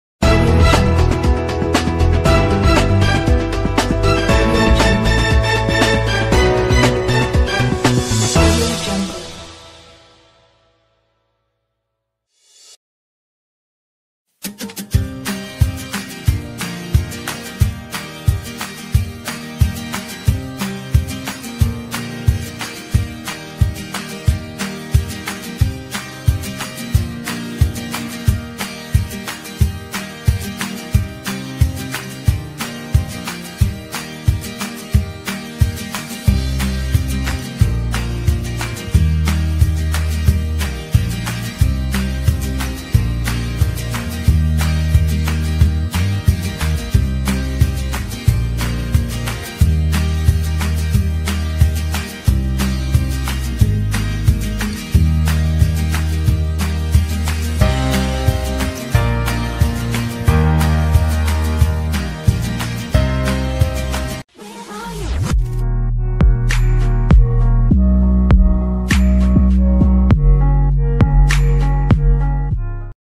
При падении военного самолета в Алжире погибли 200 человек
В Алжире упал военный самолет. Как сообщает Al-Hadath, погибли по крайней мере 200 человек, выживших в результате авиакатастрофы нет. Официальной информации о количестве жертв пока не поступало.Катаст...